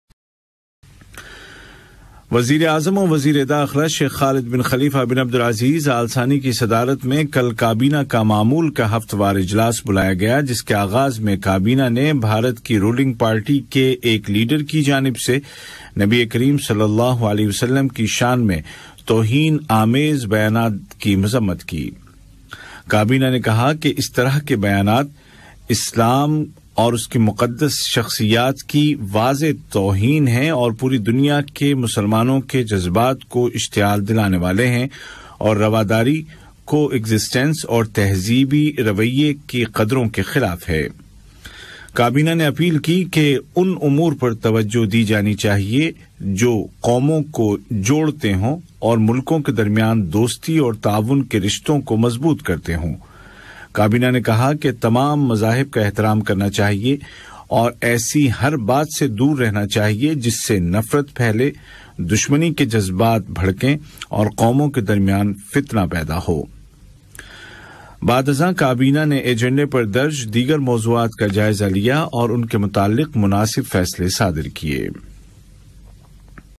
The Cabinet underscored the State of Qatar's condemnation and denunciation of the remarks
made by an official in the ruling party in India against Prophet Mohammed (may blessings and peace be upon him).